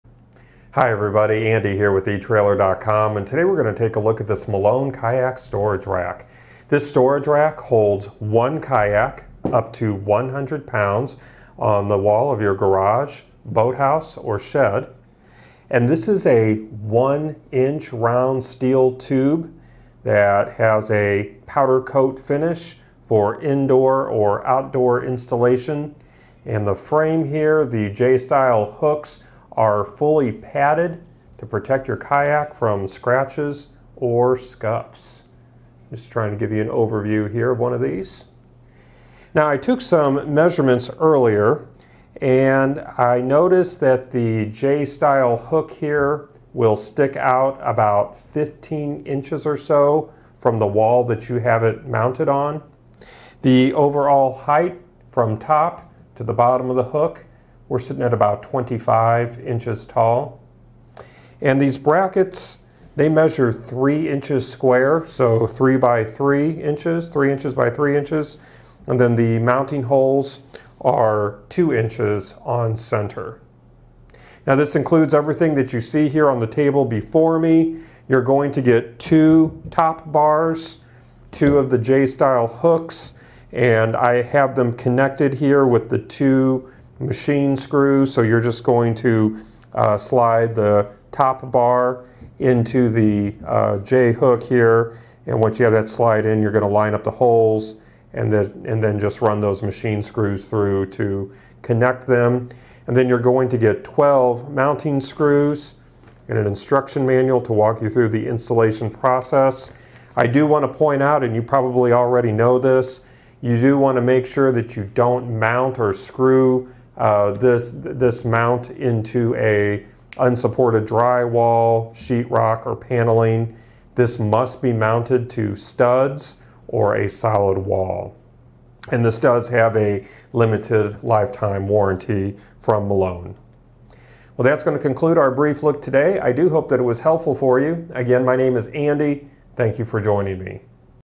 etrailer | Let’s Check Out the Malone Kayak Storage Rack for 1 Kayak
Hi, everybody.  Andy here with etrailer.com, and today we're gonna take a look at this Malone Kayak storage rack.  This storage rack holds one kayak up to 100 pounds on the wall of your garage, boathouse or shed, and this is a one-inch round steel tube that has a powder coat finish for indoor or outdoor installation, and the frame here, the J-style hooks are fully padded to protect your kayak from scratches or scuffs.  I'm just trying to give you an overview here of one of these.  Now I took some measurements earlier, and I noticed that the J-style hook here will stick out about 15 inches or so from the wall that you have it mounted on.  The overall height from top to the bottom of the hook, we're sitting at about 25 inches tall and these brackets, they measure three inches square.

So three by three inches, three inches by three inches, and then the mounting holes are two inches on center.  Now this includes everything that you see here on the table before me.  You're going to get two top bars, two of the J-style hooks, and I have them connected here with the two machine screws.  So you're just going to slide the top bar into the J hook here, and once you have that slide in, you're gonna line up the holes and then just run those machine screws through to connect them, and then you're going to get 12 mounting screws and an instruction manual to walk you through the installation process.  I do wanna point out, and you probably already know this.

You do wanna make sure that you don't mount or screw this mount into a unsupported drywall, sheetrock or paneling.  This must be mounted to studs or a solid wall, and this does have a limited lifetime warranty from Malone.  Well, that's gonna conclude our brief look today.  I do hope that it was helpful for you.  Again, my name is Andy.